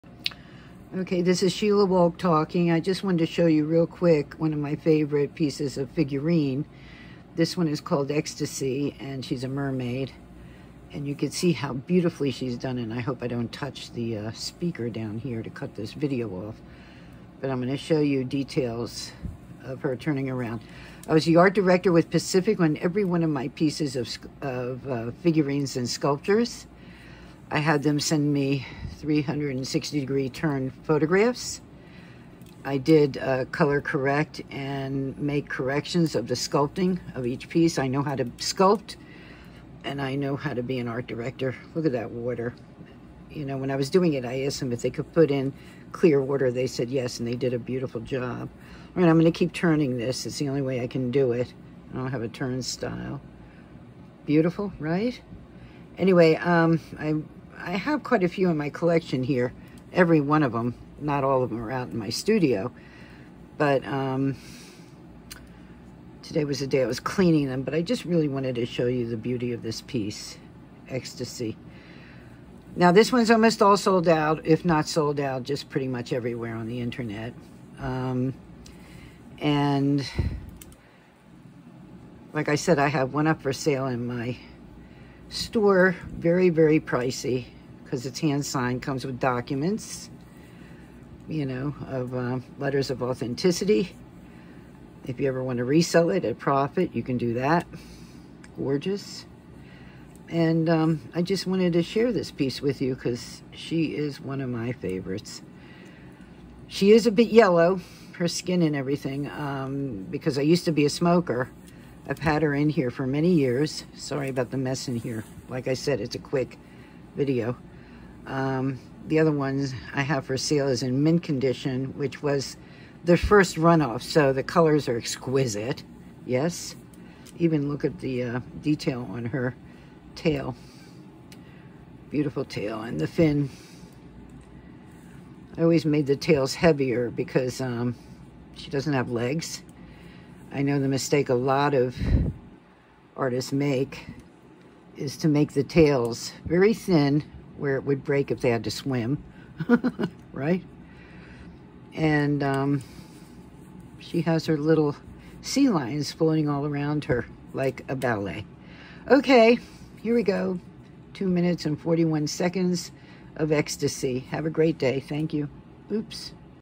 ECSTASY FiGURINE by Sheila Wolk
This is me, Sheila Wolk, showing off one of my favorite figurines named ''Ecstasy''.
:::LOOKS LIKE I SOLD OUT OF THE LAST ONE, SORRY::::
.I stopped doing figurines abouot 18 months ago or more..Hard decision to make but so be it and I have been signing many and adding to my Collectors Editions in storage facilities. Hope you enjoy thebeauty of this little mermaid, I sure did enjoy creatin g the painting andlater this figurine from it. thank you